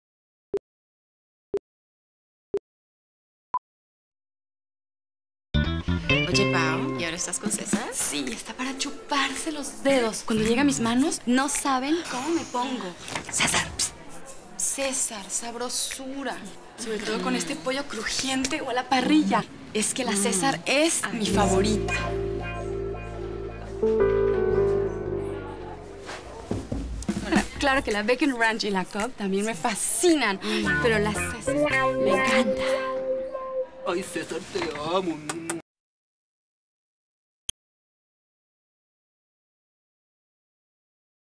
"Eaves Drop" McDonalds
Alejandro Cardenas on a McDonalds commercial for the Spanish market